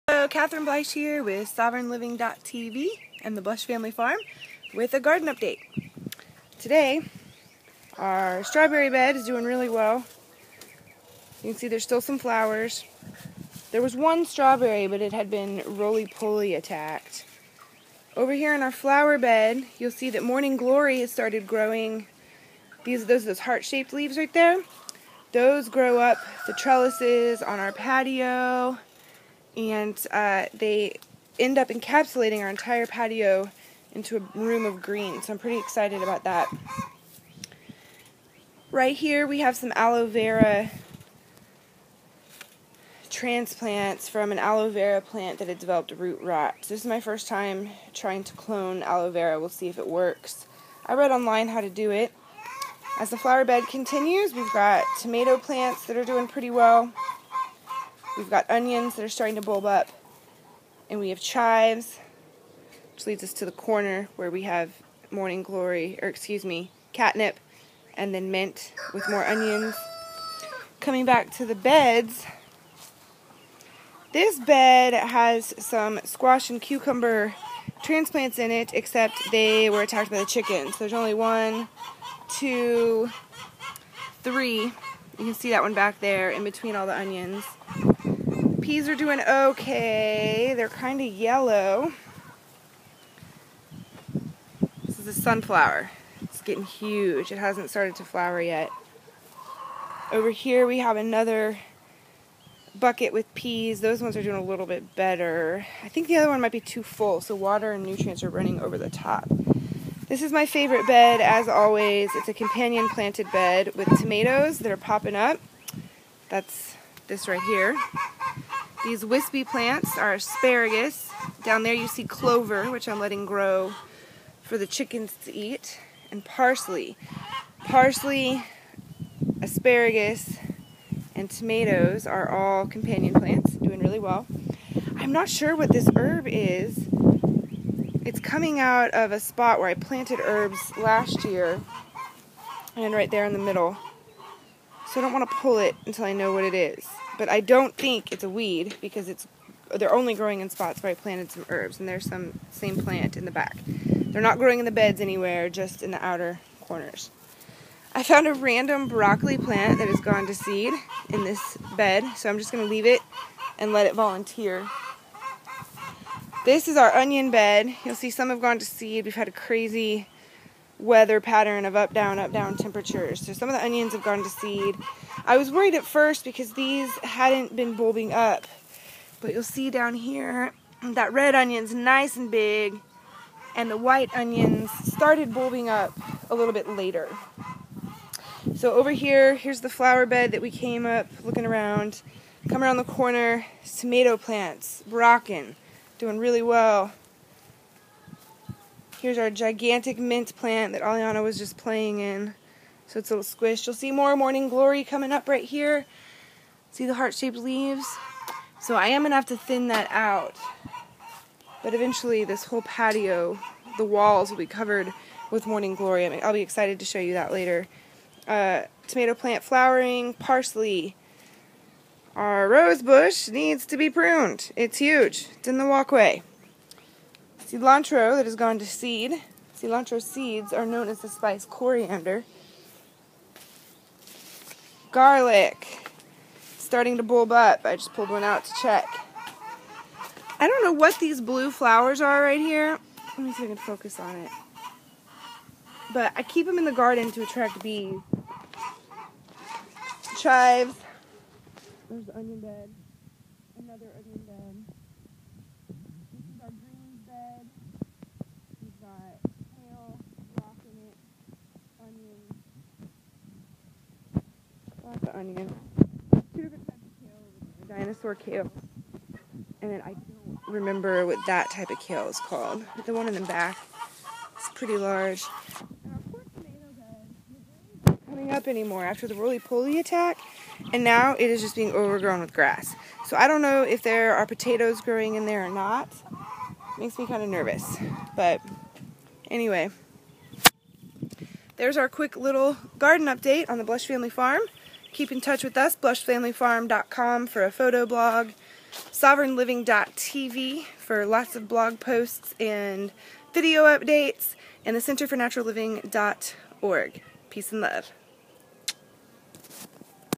Garden update on the blush family farm!
See how everything has grown. My hand covered the mic while showing the kale, sorry :/